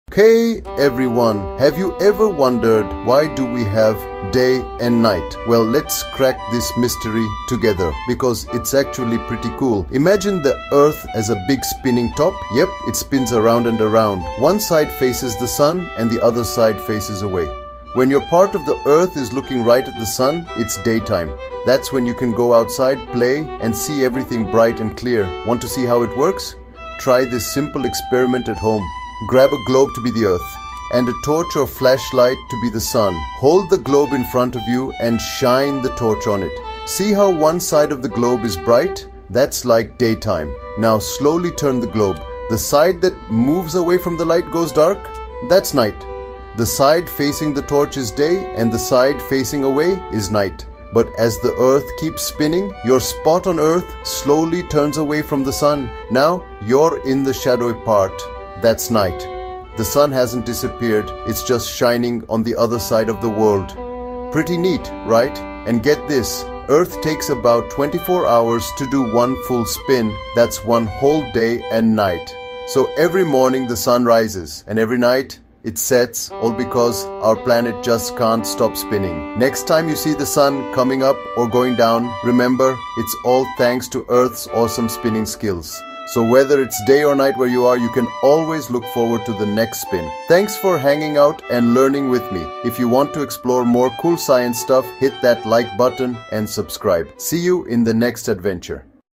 Why Do We Have Day and Night? | Easy Science for Kids
Have you ever wondered why we experience day and night? In this engaging video, we break down the simple science behind this fascinating phenomenon!

Join Pankaj as he explains how the Earth’s rotation creates the cycle of daylight and darkness, perfect for curious kids and budding scientists. With easy-to-understand visuals and fun examples, we’ll help you grasp this essential concept of our planet’s movements.